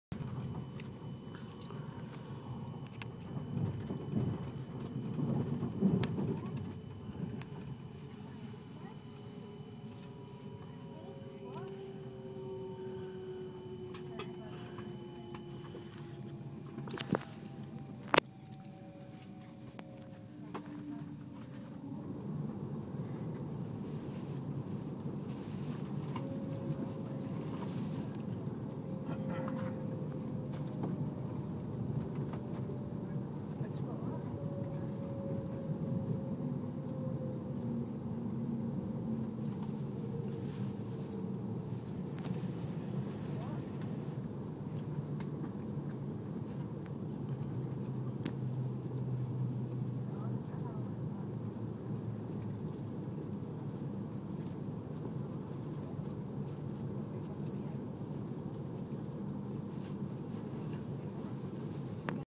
Arrival In Oxenholme VT 390 25 April
Virgin Trains class 390 Pendolino